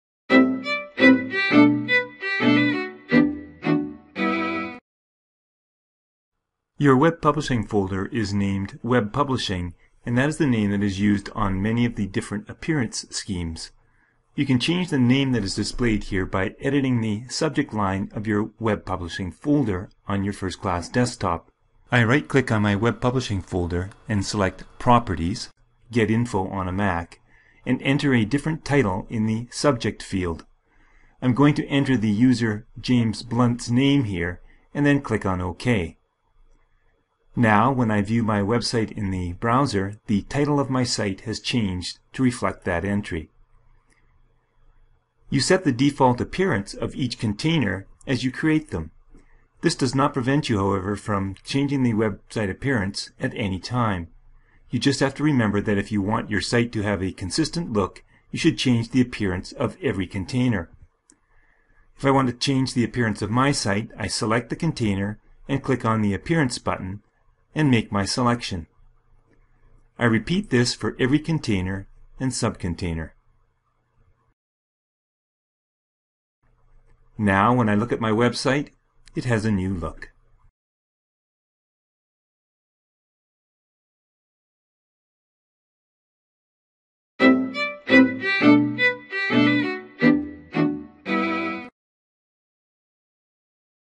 Web Publishing - Change Name and Appearance
Learn how to change the name and appearance of your Web Publishing folder and contents.